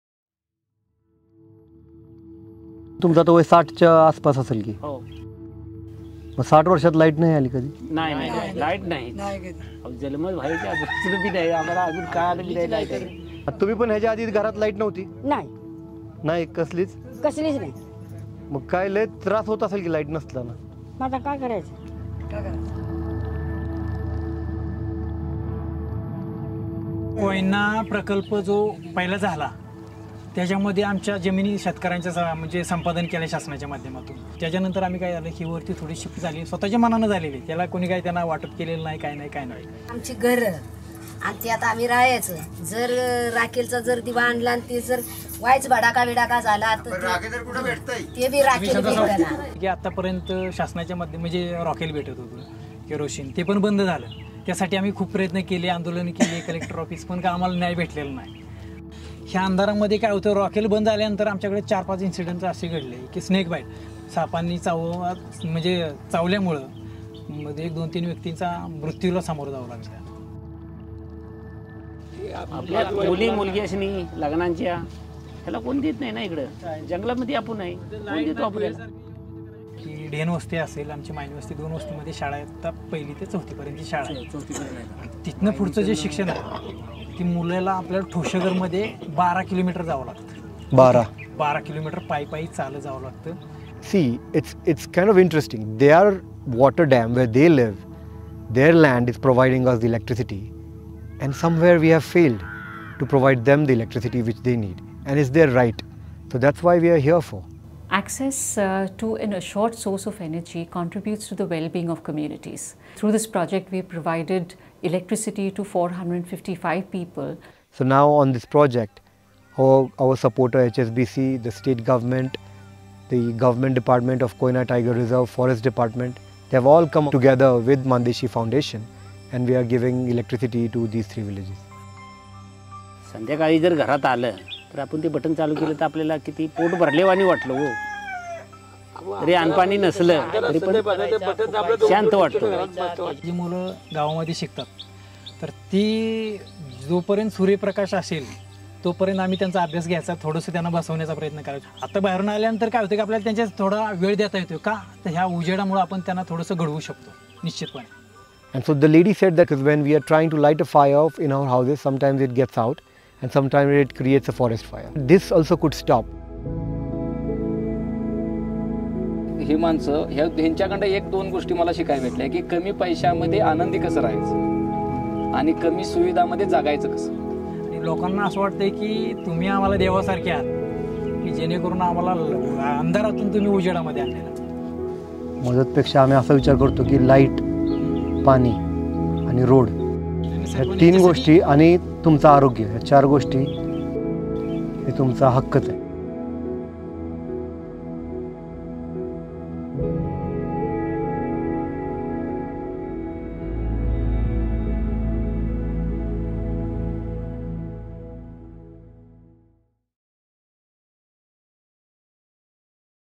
Solar Villages - Watch how solar energy provided electricity to 3 villages in Maharashtra
Did you know that there are still villages in India that do not have electricity? Villages in which people have lived in complete darkness for over 50 years. Watch how Mann Deshi Foundation partnered with HSBC India, the Sahyadri Tiger Reserve and Selco Foundation to provide 4 villages with Solar lights, Solar Fans, Solar Flour Mills, Solar Dal Mils and Solar Educational TV sets.